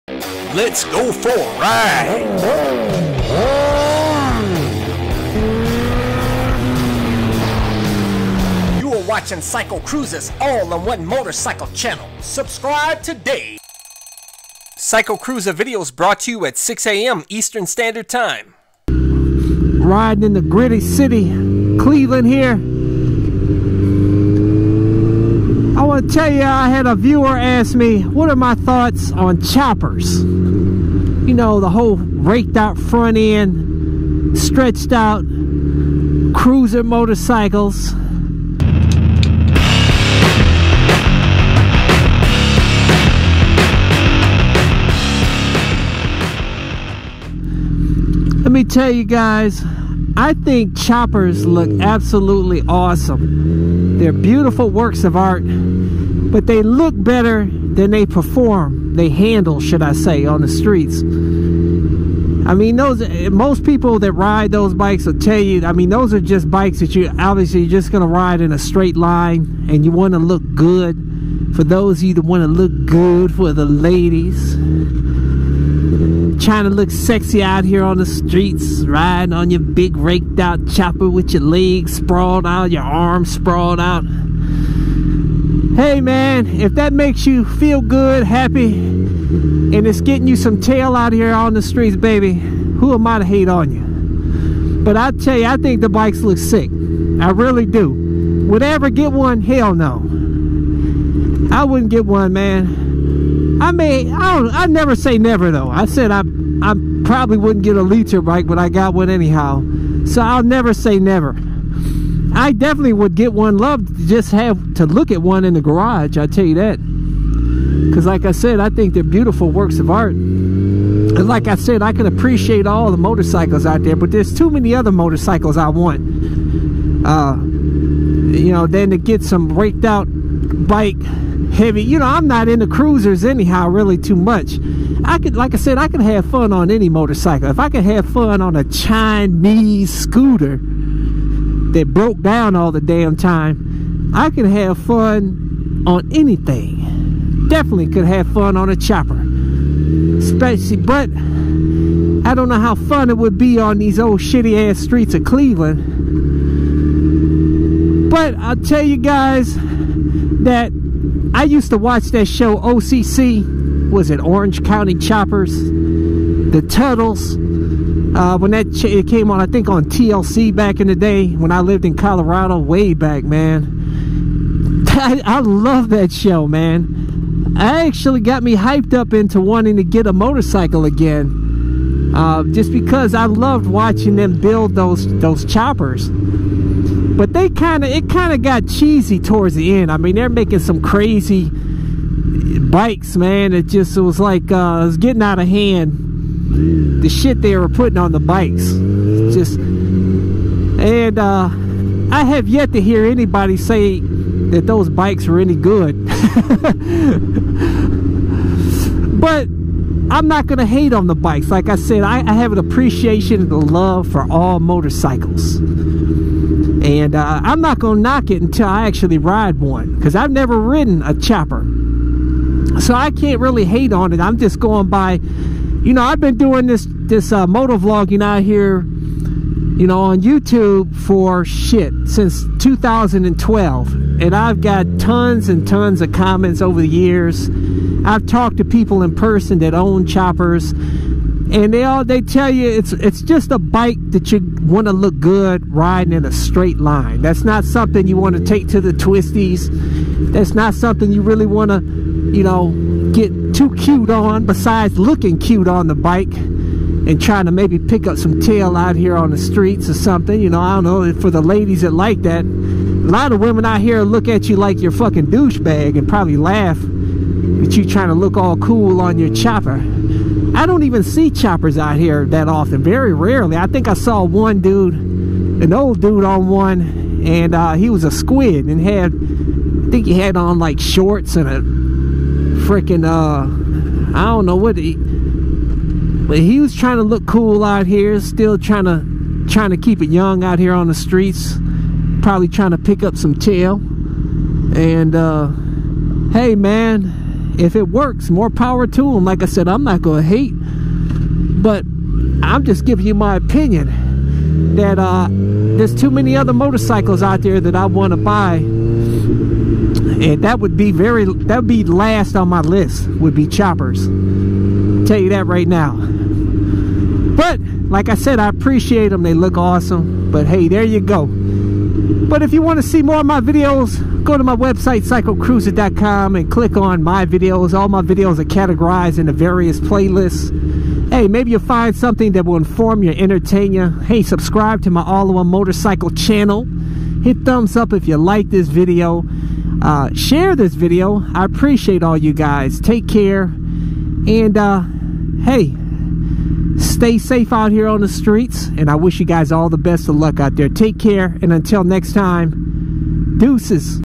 BAD Things About Chopper Motorcycles | MotoVlog
Good and bad about chopper motorcycles. Would I ever get one?